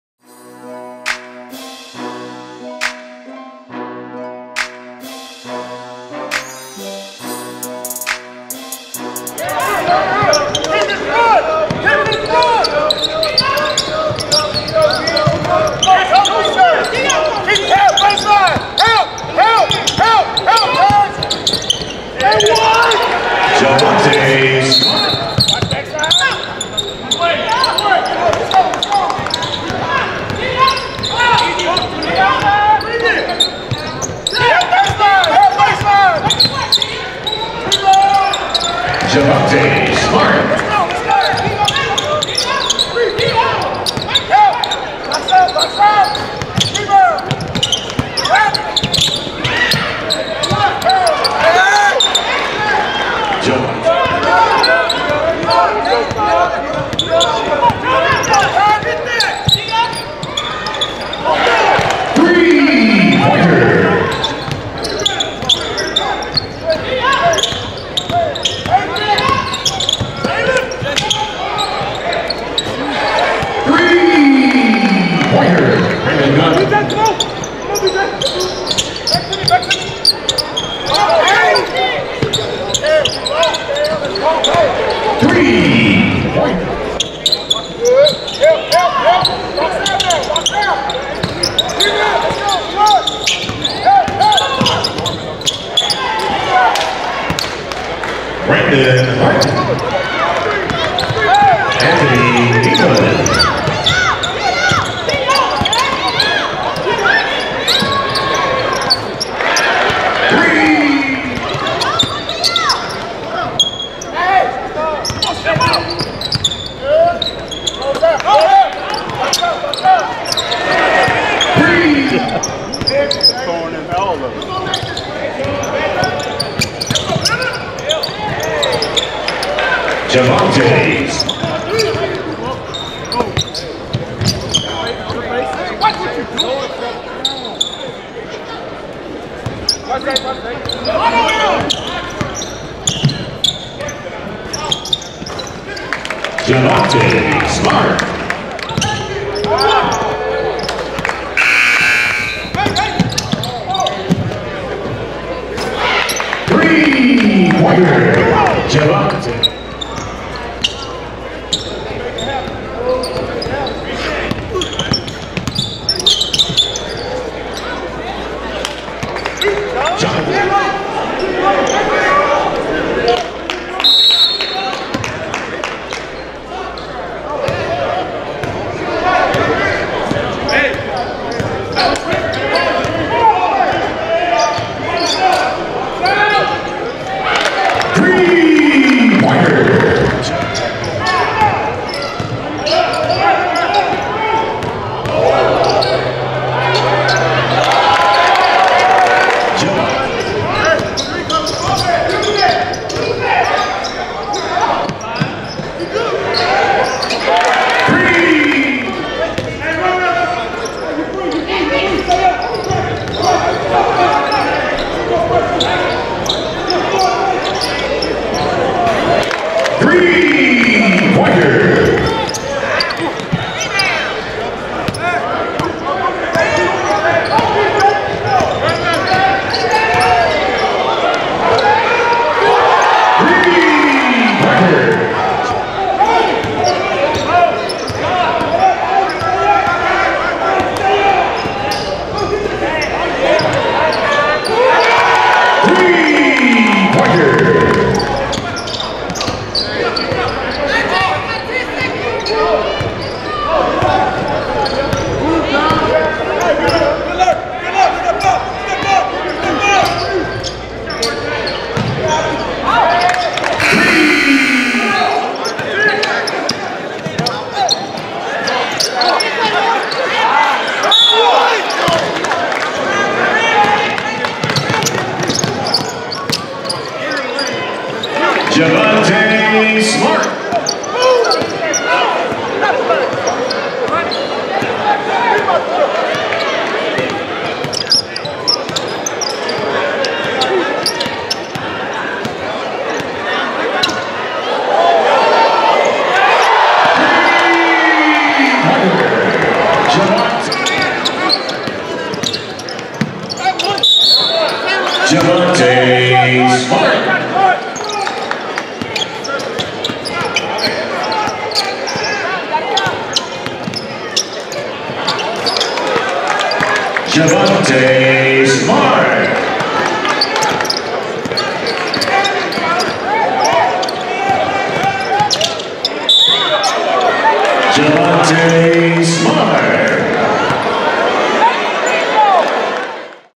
LSU Commit Javonte Smart Averaged 50 PPG In Texas At The Thanksgiving Hoopfest!!
I'm part time vlogger/part time sports videographer